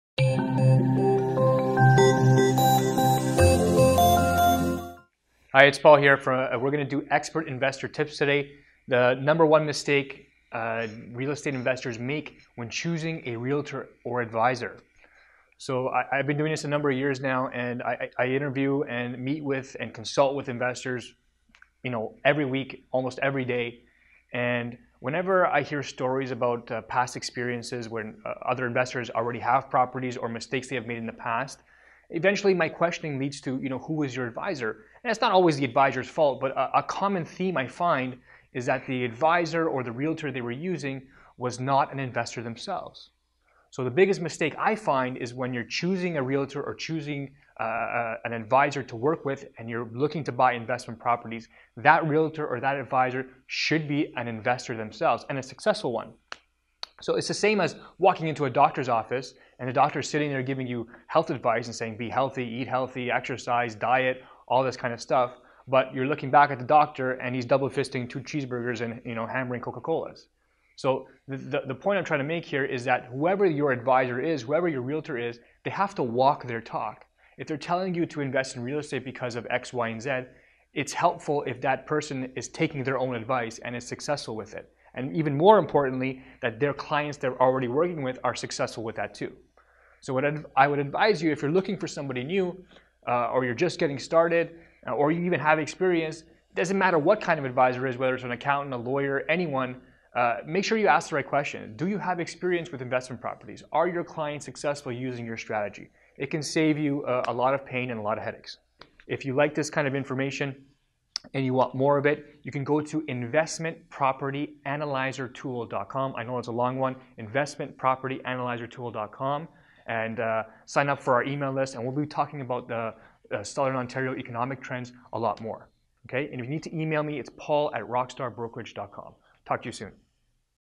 Explained: The True difference between Realtors & Investment Advisers?...and how to choose?
This video explains the difference between your average Realtor and working with an Expert Real Estate Investment Advisor… and how to make the right choice

A true advisor must always “Walk Their Talk”

Another way to say that would be, “A true advisor put their money where their mouth is”

Maybe I’m tooting my own horn here, but I have to be honest…

I believe myself to be a very successful real estate investor and I have the results to prove it.

There I said it.

I’m going to teach you everything I know and it’s going to work because I’ve already done it, and the over 200 investors I coach have already done it also…

Do you get what I’m trying to say here?

Register. for my next to sample a bit of our flavor